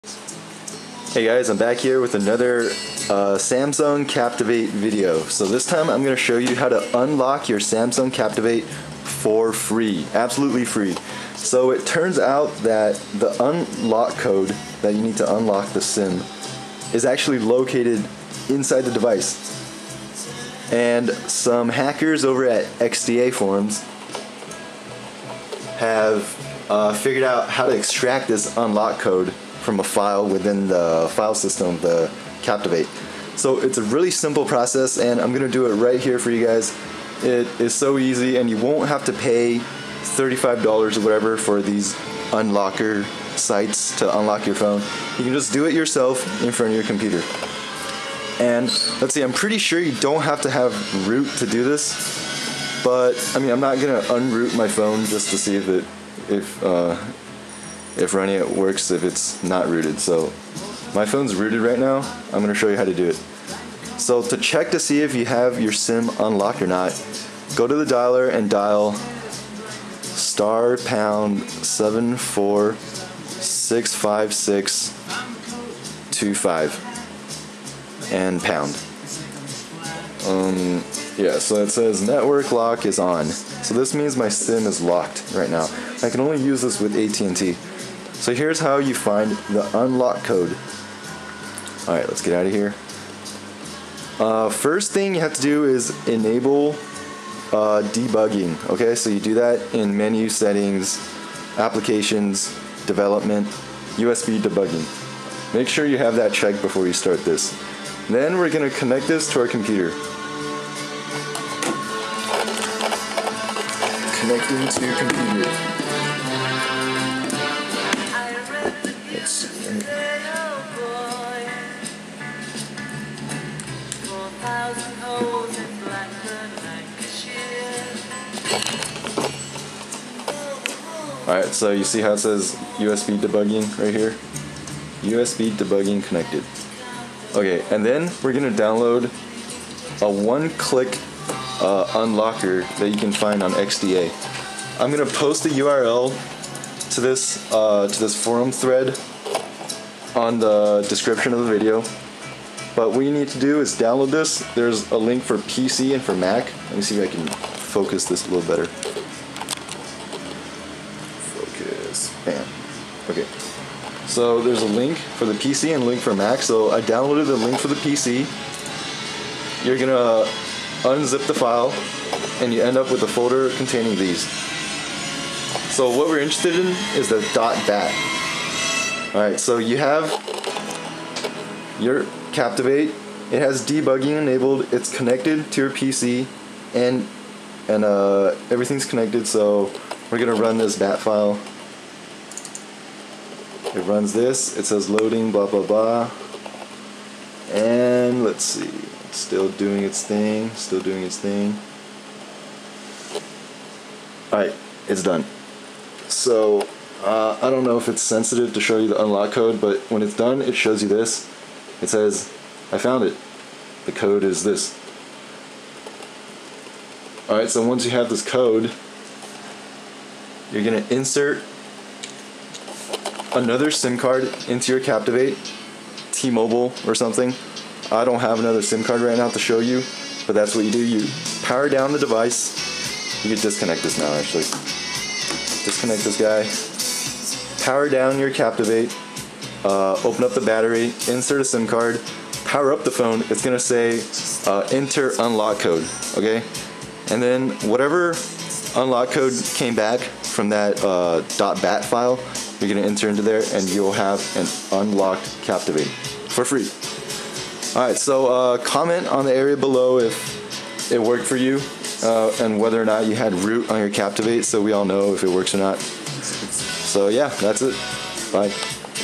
How to Unlock Your Samsung Captivate (AT&T) for FREE
Update: Search the Market for Unlock Captivate, and download the free app by DaGentooBoy! I have personally tested it, and the Unlock code that it gives me is the same as the other harder methods. I have no clue what the Unfreeze code does.

Why is unlocking useful? You can use your smartphone on other carriers. For example, use T-Mobile, or a prepaid SIM card (gophone), or even use carriers in China when you're off traveling (theoretically).

Usually, unlocking requires you to send an "unlocker" your IMEI # and some payment around $35, but with this hack, you can get your phone unlocked yourself for free! All thanks to the folks at XDA Forums.
This video was made a while ago, when the Captivate still had Android 2.1. Do not be surprised if it does not work anymore.

I suggest checking XDA Forums for help and newer techniques to unlock.